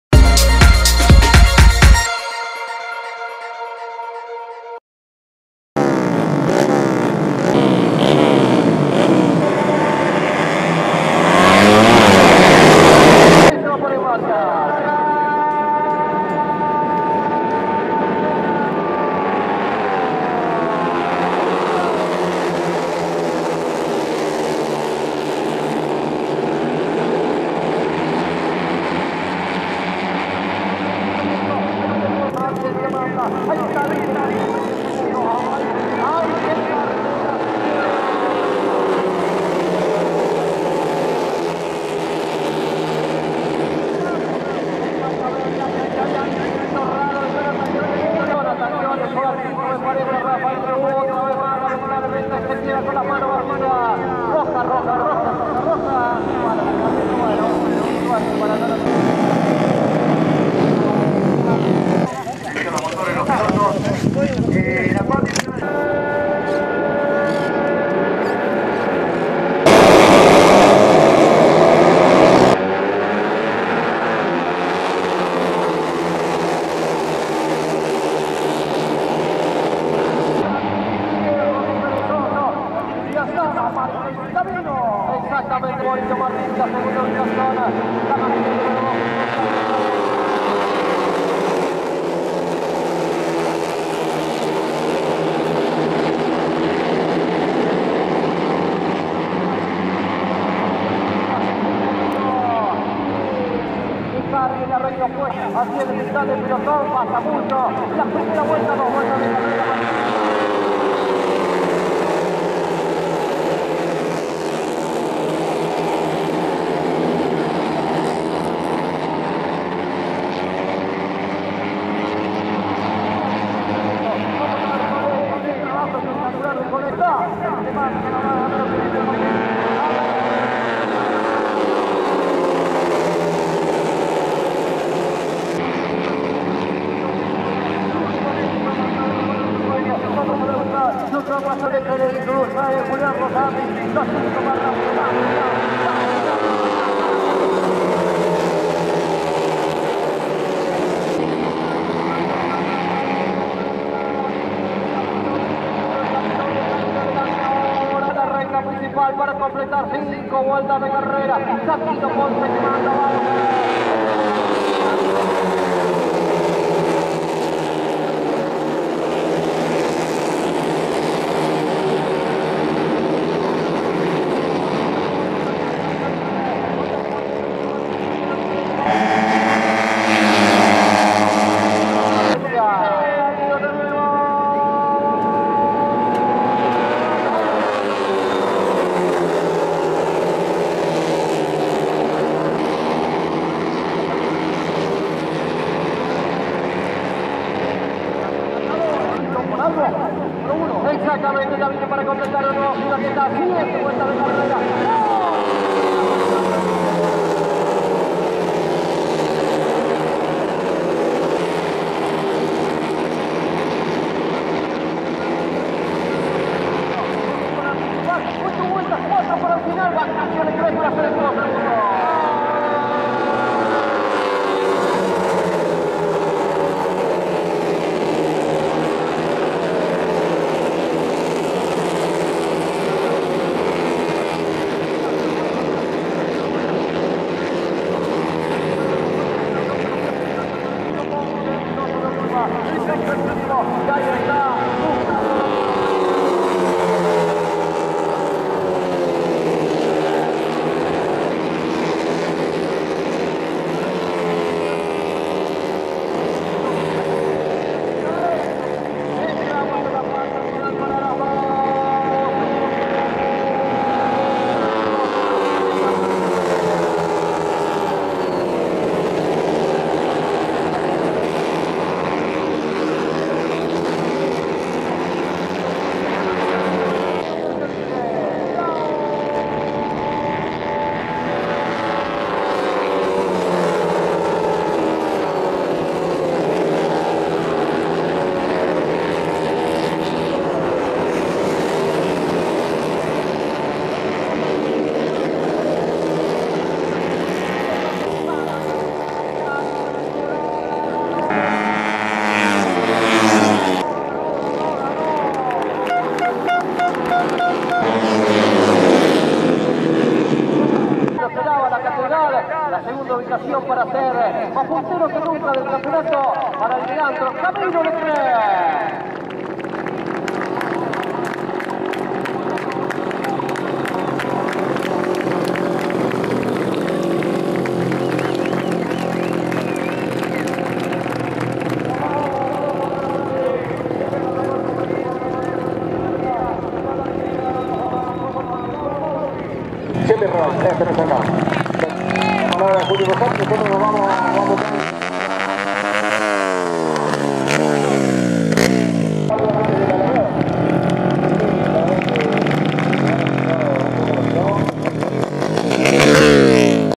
CAM 2019 - FINAL 110 Menores - 8° Fecha - Santa Clara - Santa Fe
CAM 2019 - FINAL 110cc 4T Menores - 8° Fecha - Santa Clara de Buena Vista - Santa Fe - Circuito "David Dondoni (h) de Santa Clara de Buena Vista.-